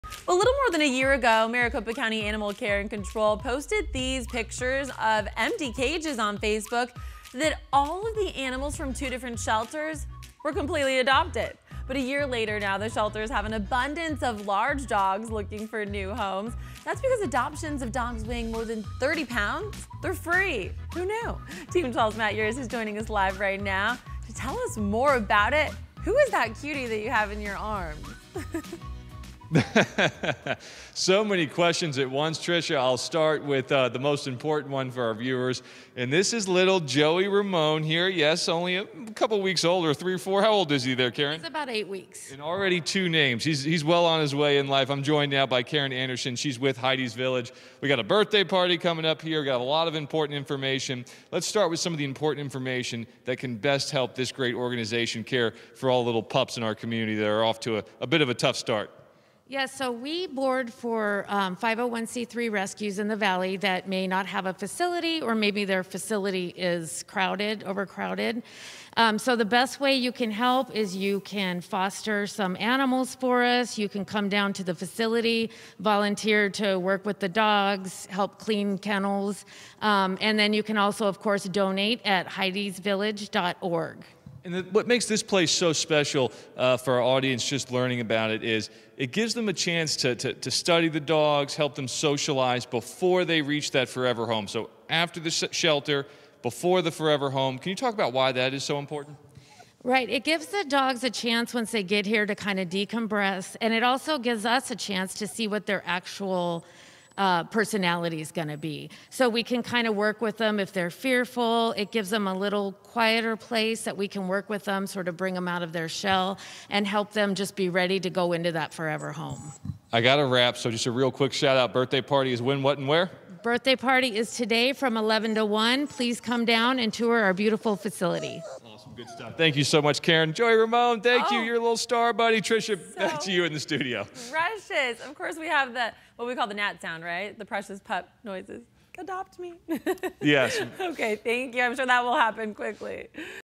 Heidi's Village animal shelter offering free adoptions for dogs above 30 pounds
The shelter boards for animal rescues in the Valley that may not have their own facility or an over-crowded facility.

If you're interested in helping, you can volunteer, foster, or donate to the shelter at heidisvillage.org